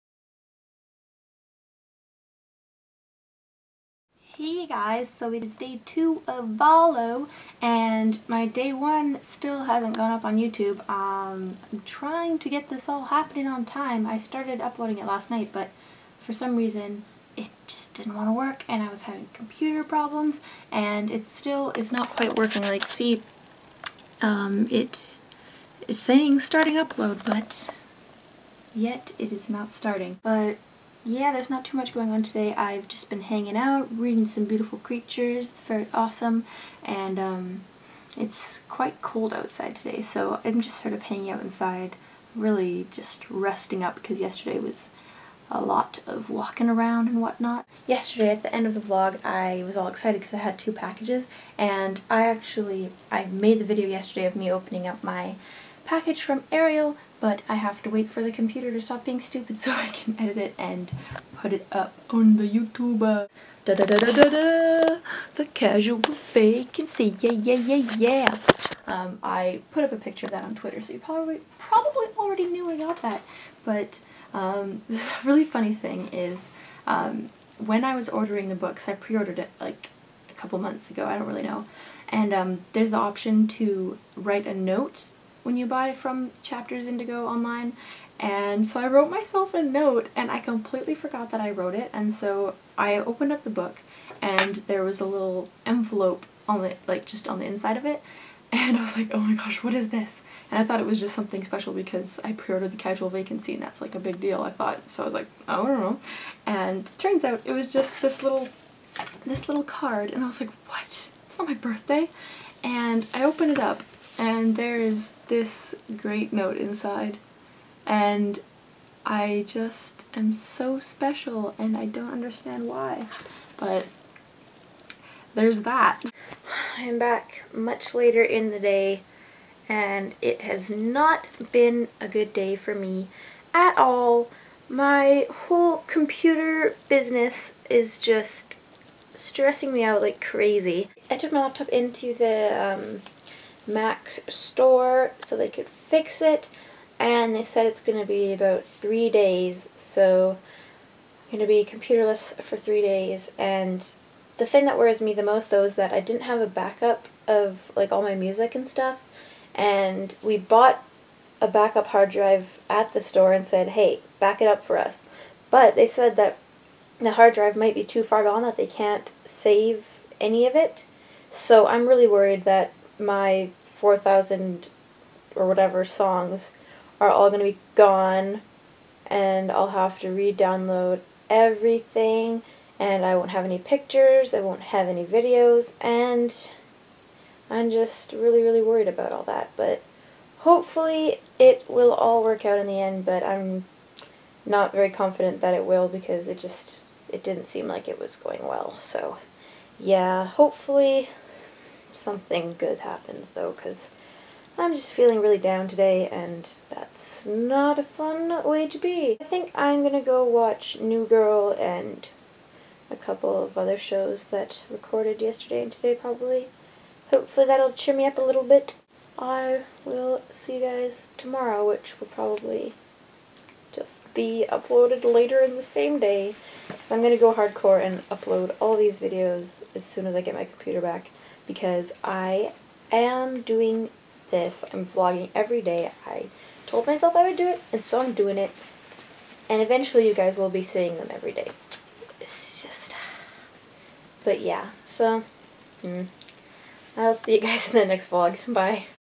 VALO DAY 2: Computer is Broken.
Day 2 of Vlog A Lot in October!
So, I finally have my computer back! These are the daily vlogs I've been doing (LIKE EVERY DAY. CRAZY, RIGHT?), and you'll be seeing many more until I get back up to date!